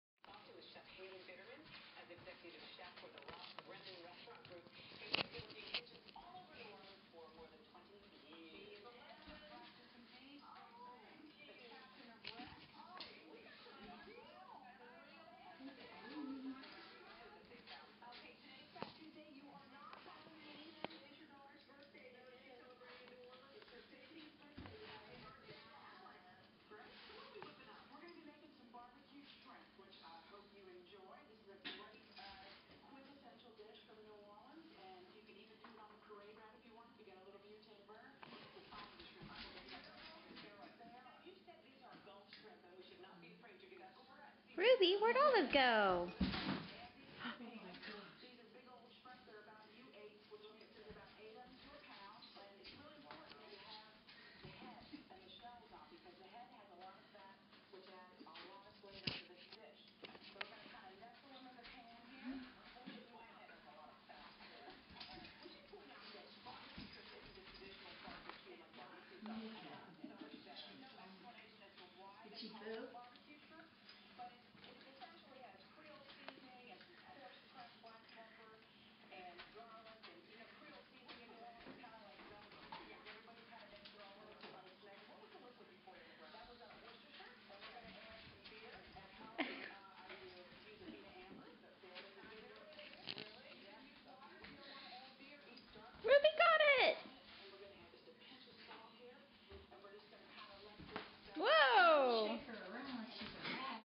Tug o War!
Olive and Ruby playing with the same toy.